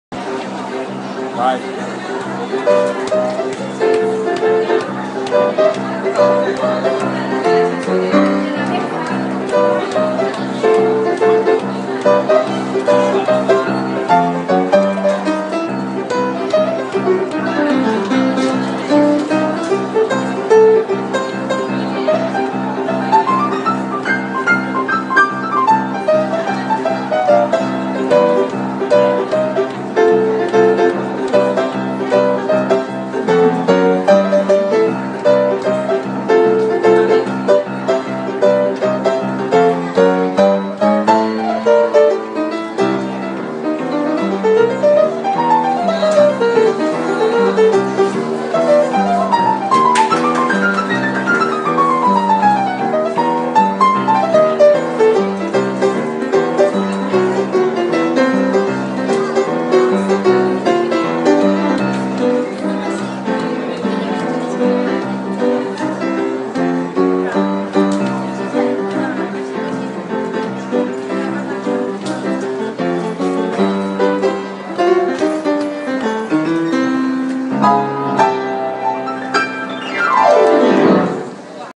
What if Van Halen played swing?
Luca Pappalardo - Piano improvisation on a free piano in Florence